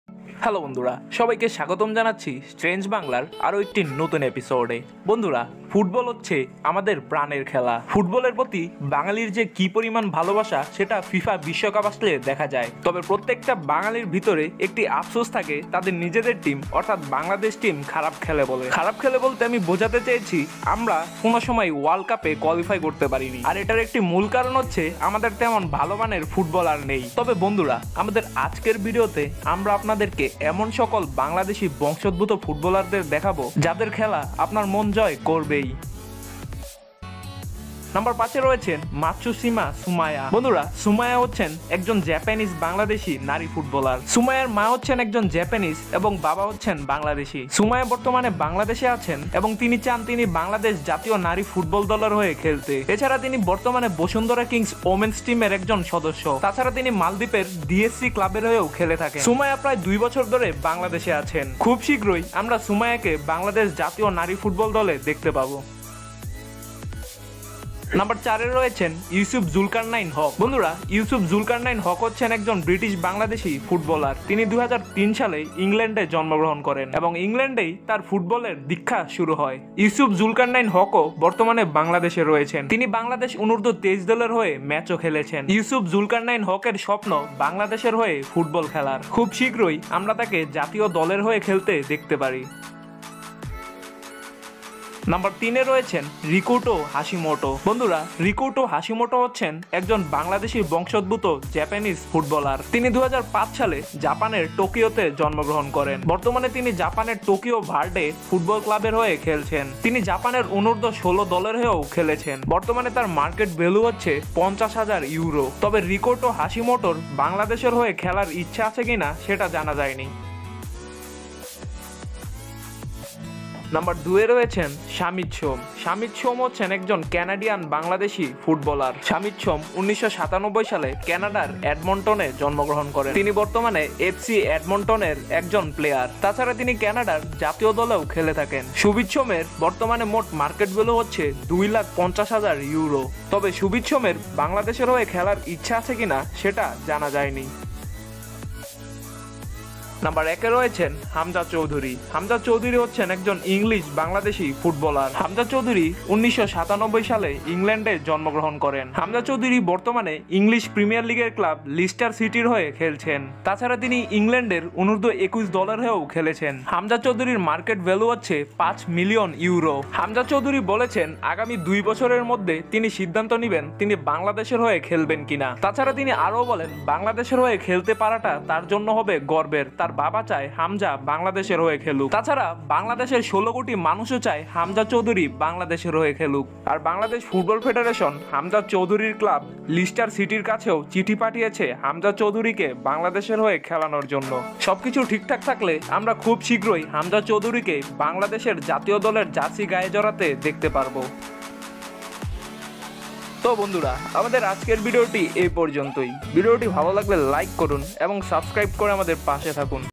সেরা ৫ জন বাংলাদেশি বংশোদ্ভূত ফুটবলার | Bangladeshi Origin Footballers | Hamza Choudhury
বন্ধুরা আমাদের আজকের ভিডিওতে আমরা সেরা ৫ জন বাংলাদেশি বংশদ্ভুত ফুটবলার সম্পর্কে জানিয়েছি

Strange Bangla